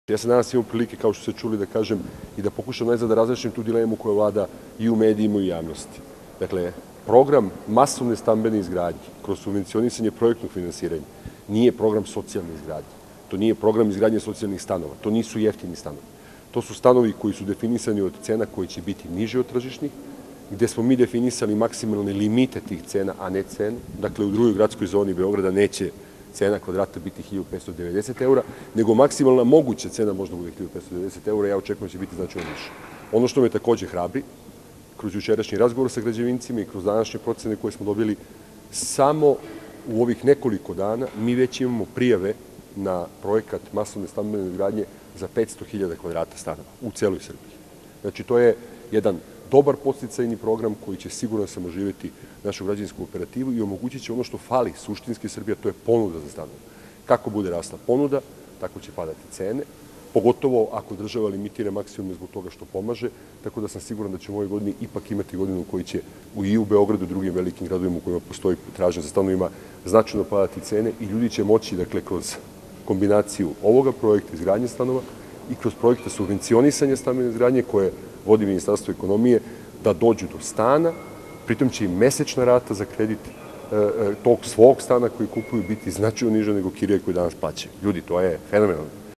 Dulić o projektu masovne stambene izgradnje (2/2)
(Beograd, 11. februara 2010) Ministar životne sredine i prostornog planiranja Oliver Dulić nakon učešća na konferenciji "Pristup socijalnom i dostupnom stanovanju — razmena iskustava o stambenim politikama u Srbiji i drugim zemljama u Evropi". (za više informacija posetite